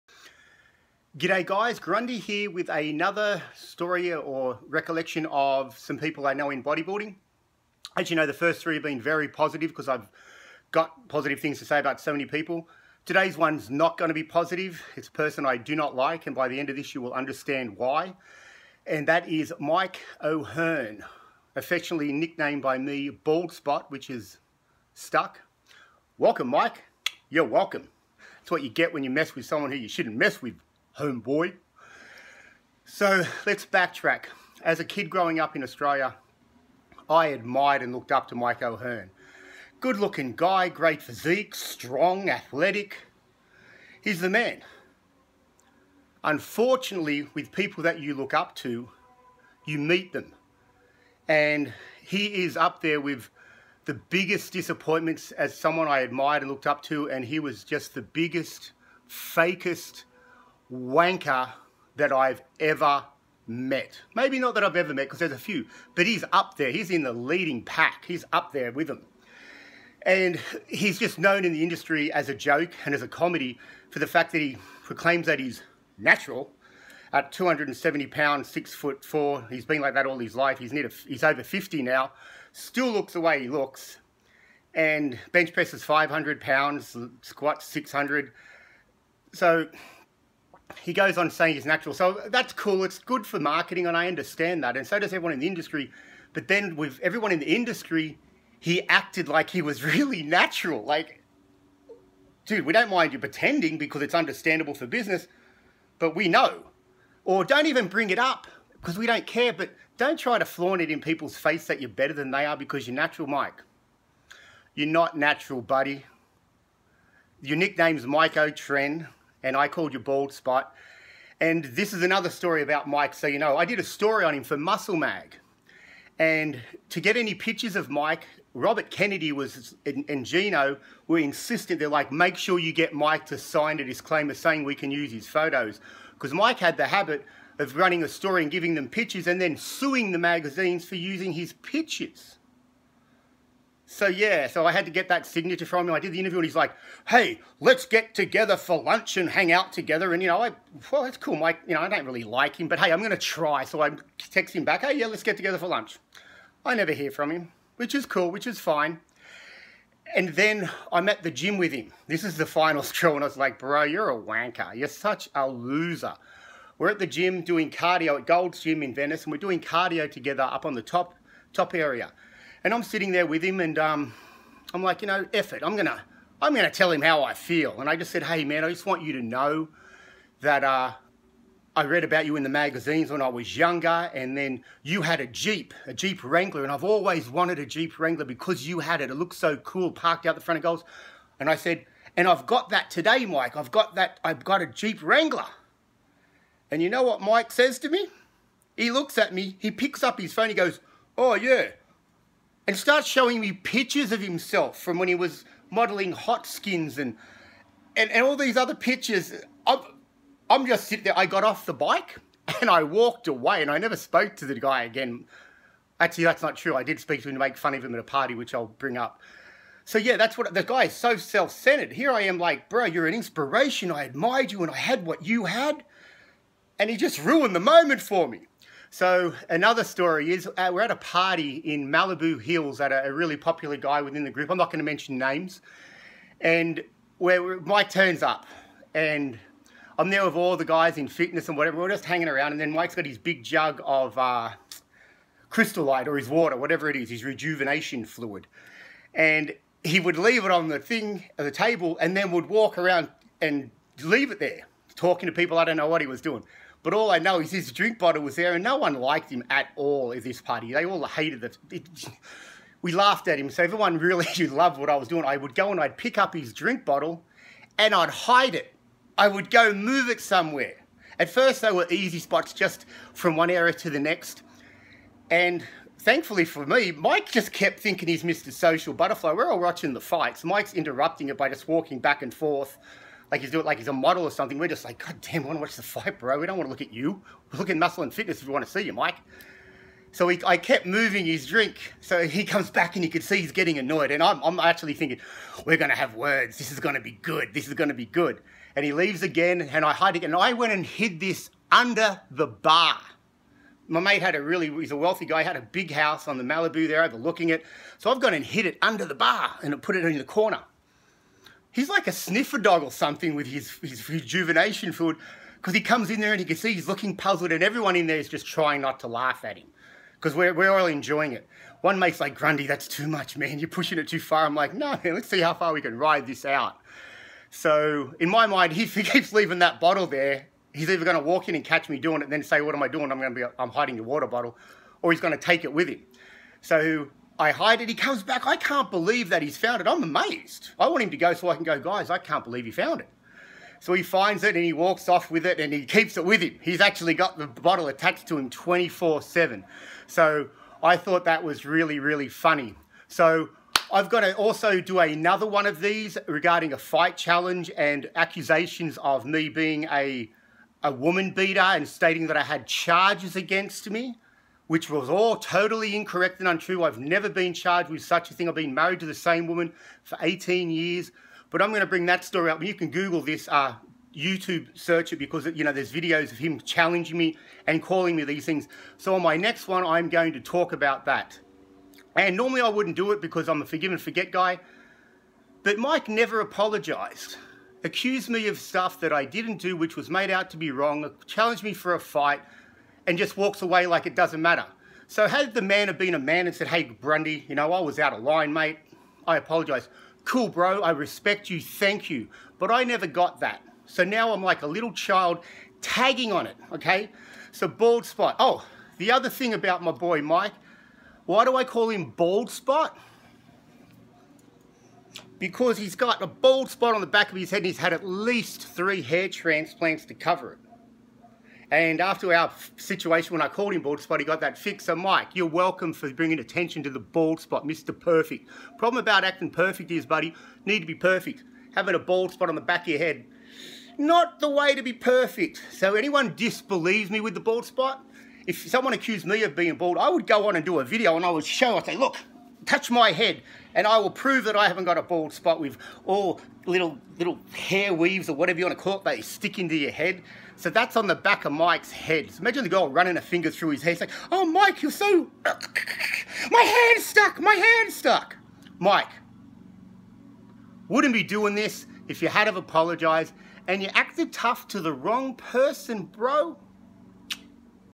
Grundy's Bodybuilding Stories! Mike O'Hearn, The Sports Biggest WANKER. Good Old BALD SPOT!!!
Former Mr. Australia Guy Grundy shares his stories from his Day's as a Bodybuilder from Sydney, Australia to Los Angles, California! In this Episode Grundy talks About Mike O'Hearn the Sports Biggest WANKER & the most Fake person you will ever meet. I used to Admire & Look up to this Bloke & then I met him & he was a MAJOR DISAPPOINTMENT as a Man & Fitness Celebrity. Sometimes meeting your Heroes is a Bad thing. I have NEVER met such a Dickhead in all my life. I share some stories on who Mike is and how he acts!